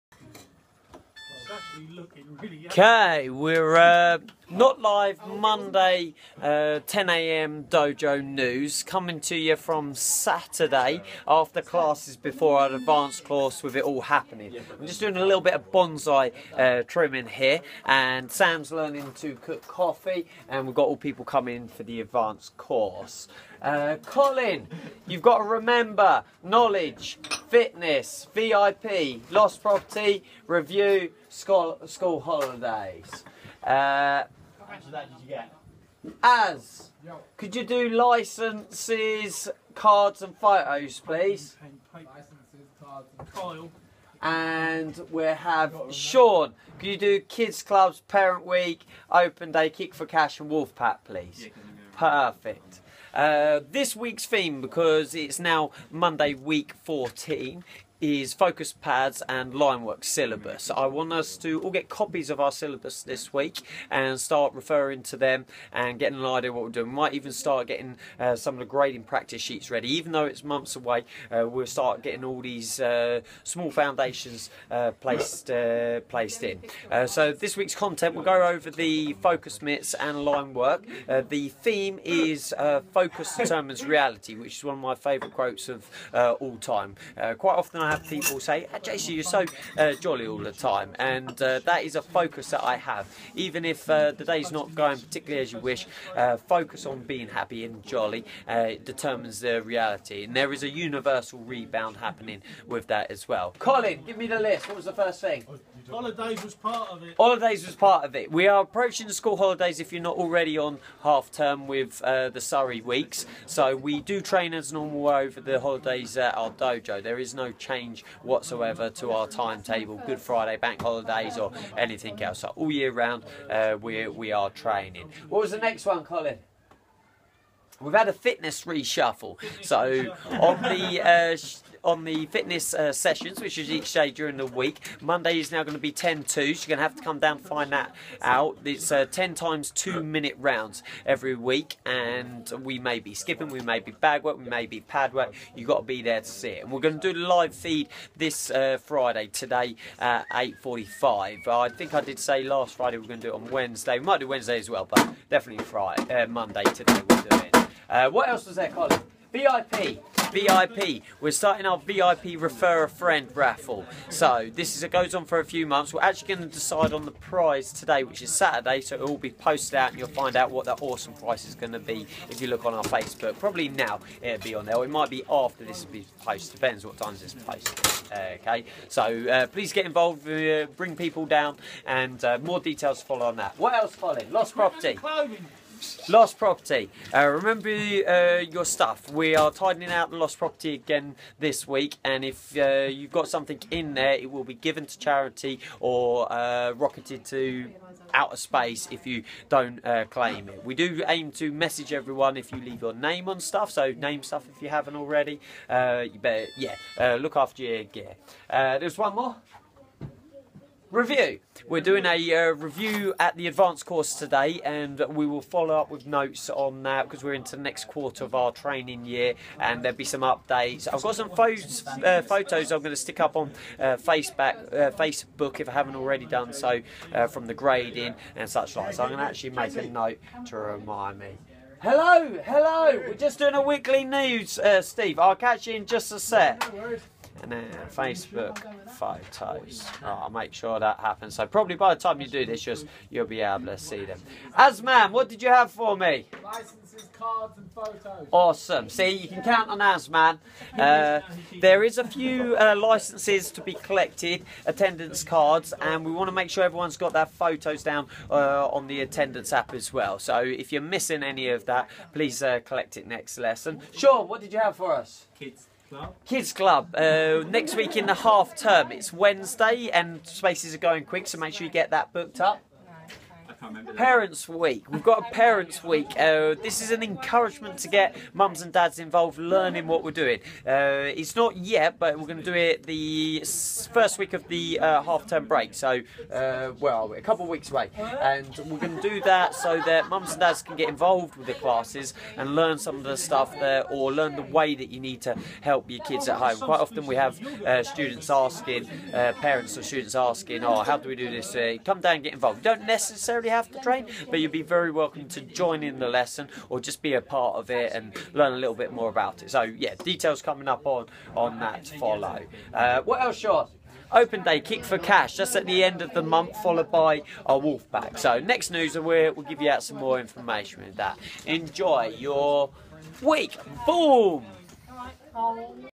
News @10am Weekly Dojo Message
Focus Mitts / Linework (Syllabus)
Your focus determines your reality.

Knowledge (YouTube)
Fitness shuffle
VIP
Lost property
Review updates (FB Photos)
School Hols

VIP
Licences
Attendance cards
Attendance photos

Kids club
Parent week
Open day & Kick4cash
Wolf pack